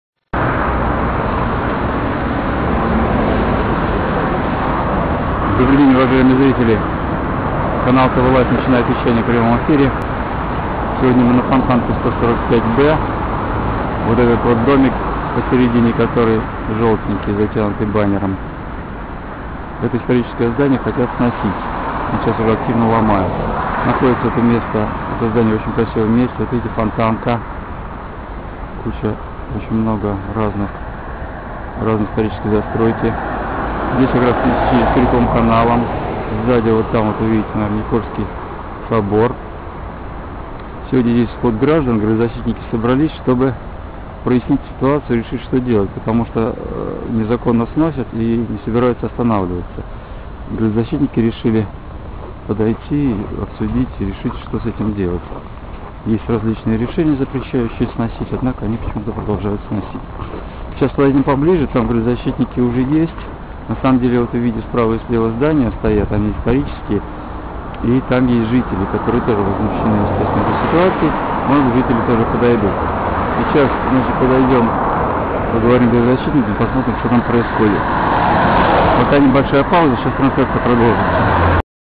01 Сход граждан на Фонтанке 145 Б. Петербург. 07.02.12.
Вид с другого берега Фонтанки.

На Фонтанке 145 Б незаконно сносят историческое здание, дом Зыкова. Уже незаконно снесены внутренние флигели. Градозащитники опасаются за лицевой флигель.

Градозащитники собрались на пикет, чтобы помешать варварам уничтожить здание, и чтобы обсудить меры по спасению исторического памятника. В акции приняли участие простые граждане, активисты Живого города и группы ЭРА.

Retired people on the Fontanka 145 B. St. Petersburg.